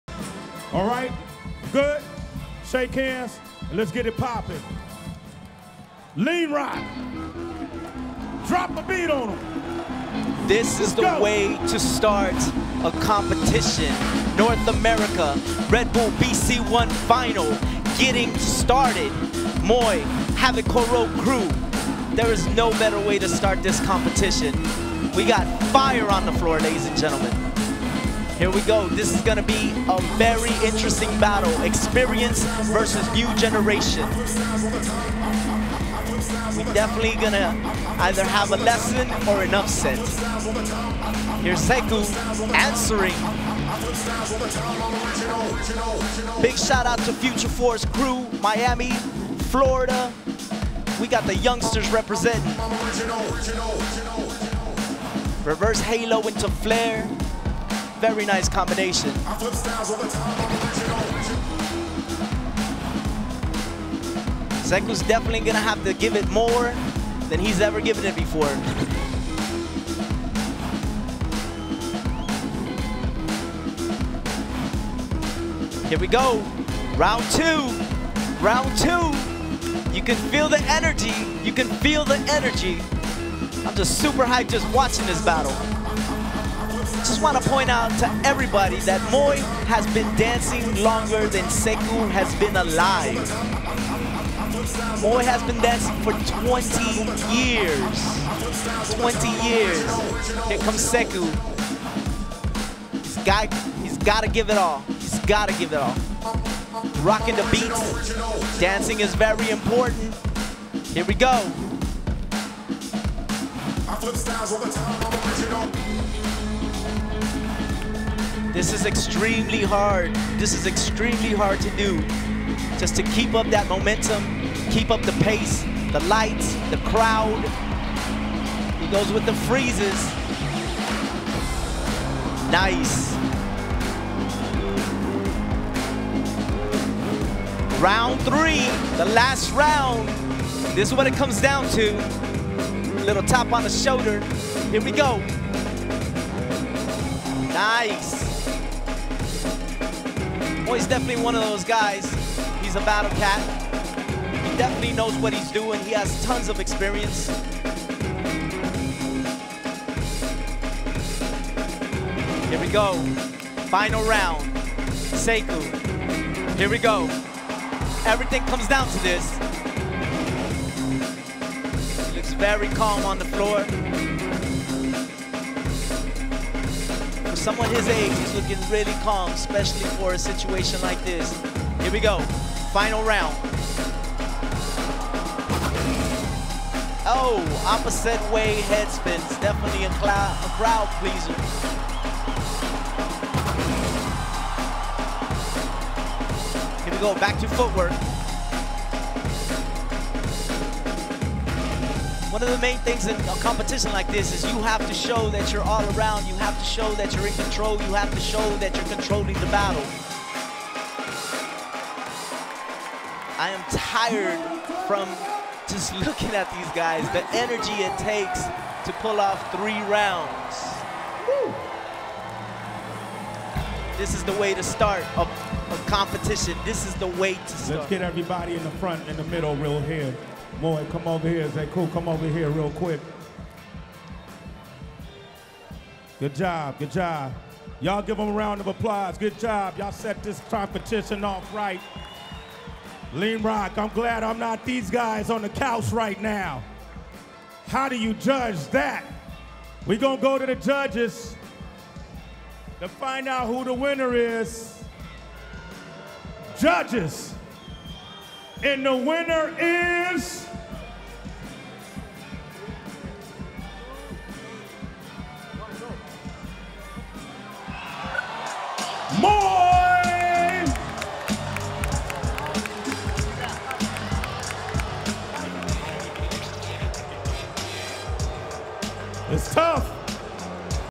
Moy VS Zeku - Red Bull BC One North American Final 2015
Moy and Zeku face-off in the 2015 Red Bull BC One North America Final.